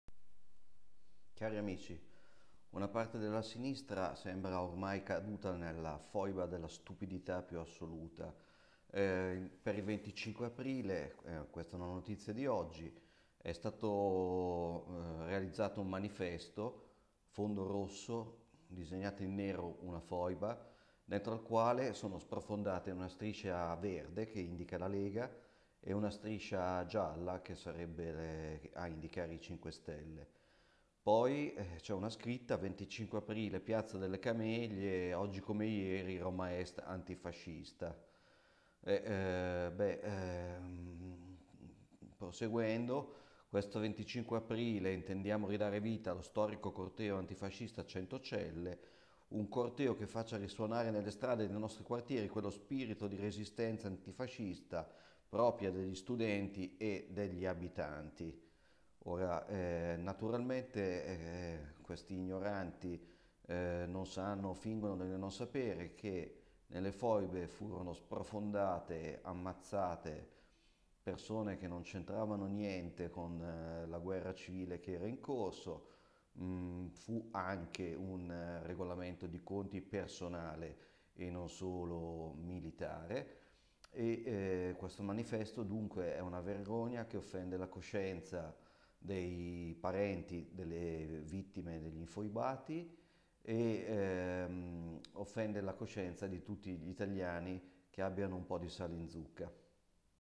25 aprile, quei manifesti vergognosi
Alessandro Gnocchi commenta manifesti sui muri di Roma per il corteo del 25 aprile